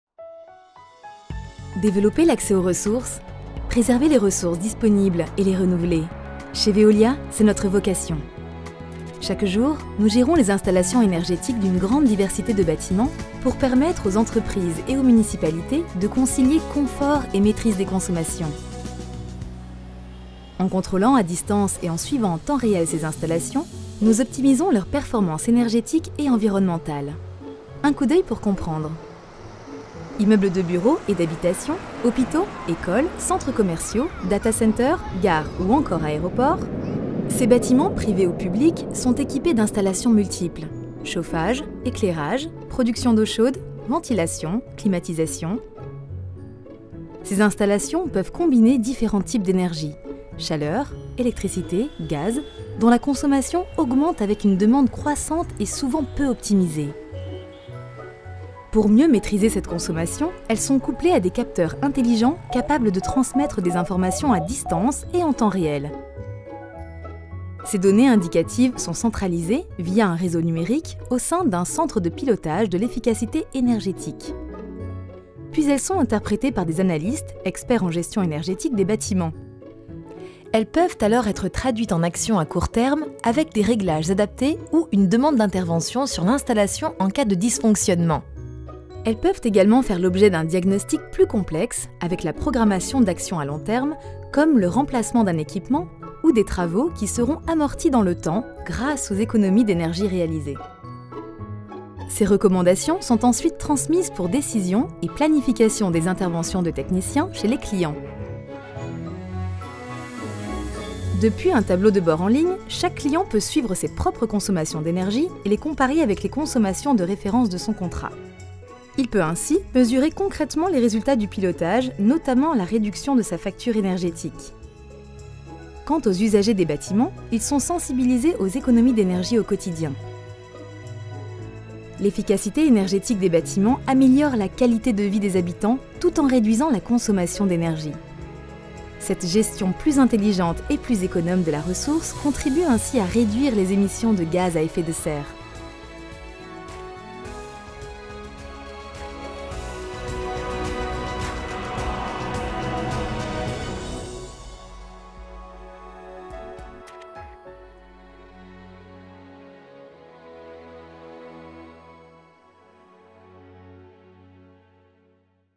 L'efficacité énergétique des bâtiments | Veolia
Maintenance des installations, gestion en hypervision et maîtrise des consommations ; la performance énergétique des bâtiments expliquée en 3 minutes.
: Nos autres solutions pour les villes et les industries.

2016 - Direction de la communication Veolia
Production : Benoit de La Rochefordière
Agence Comçi-Comça :
Rédaction : Amélie Blanquet
Graphisme 3D : François Berthemet